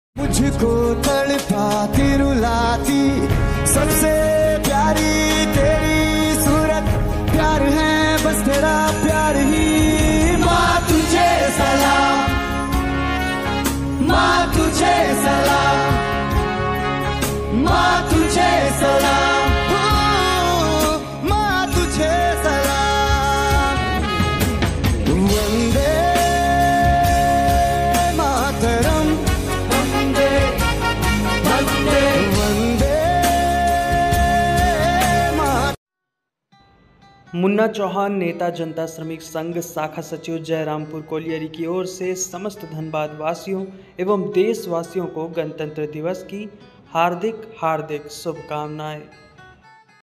मुन्ना चौहान, नेता,जनता श्रमिक संघ शाखा सचिव जयरामपुर कोलयरी की ओर से गणतंत्र दिवस की शुभकामना
मुन्ना चौहान, नेता,जनता श्रमिक संघ शाखा सचिव जयरामपुर कोलयरी